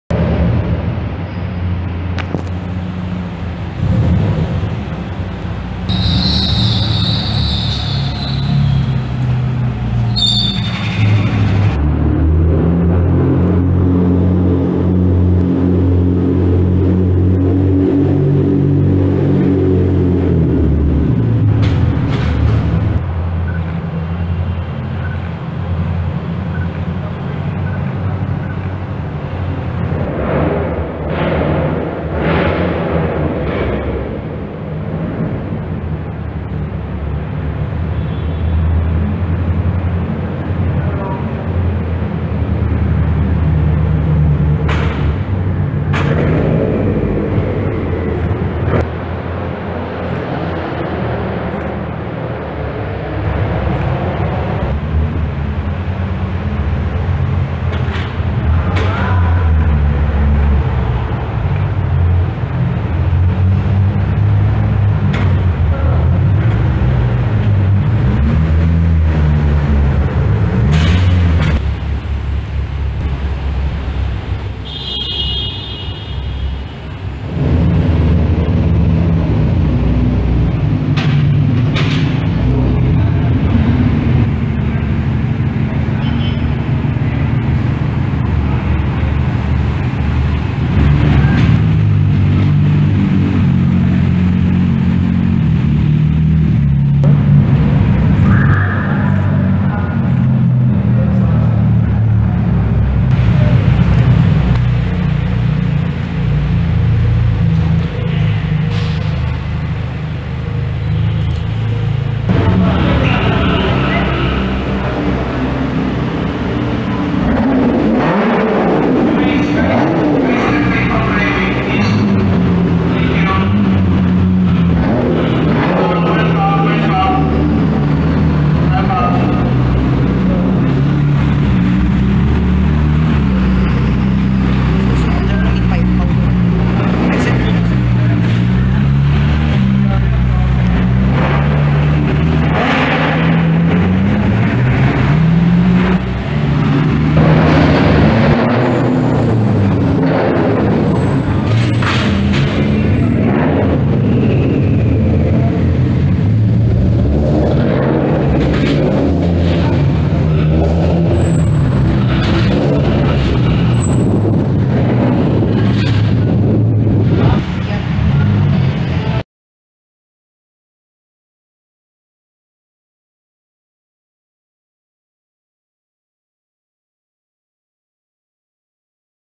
Sportscars Arriving - WRX STI, 458 Italia, Silvia, Evo's & MORE!
Street Customs PH held the Endless, a car meet during the night of June 2, 2018. The location of the car meet was the 6th floor of the parking structure in Mall of Asia Annex (MAAX) in Pasay City, Metro Manila (NCR). Most of the cars that were there are JDM tuners, but there were also some non-JDM cars such as the Ford, BMW, Porsche, and the Ferrari. This doesn't feature all the cars that went to the car meet because I had to leave earlier than the meet's end time due to some circumstances :( .